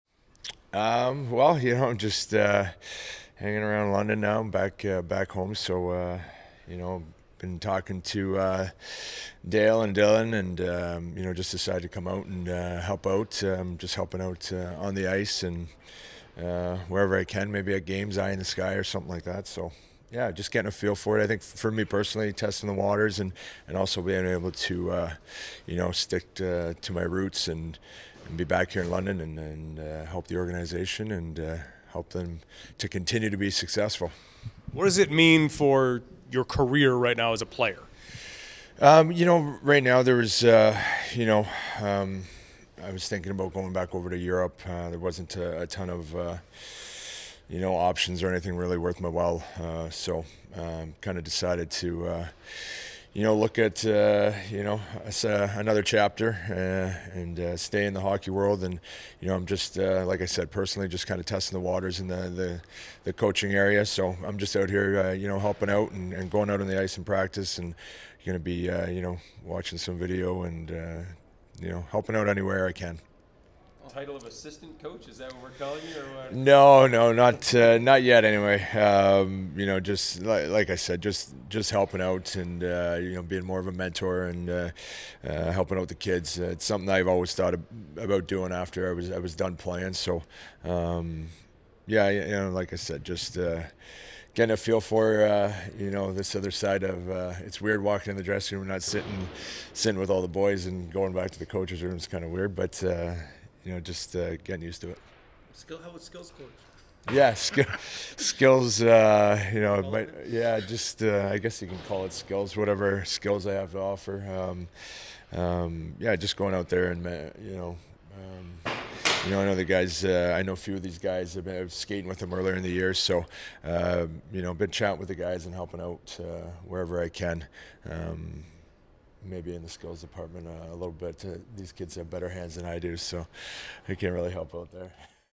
Brandon Prust is back with the London Knights
After more than 450 games in the National Hockey League, Brandon Prust is "testing the coaching waters" with the London Knights.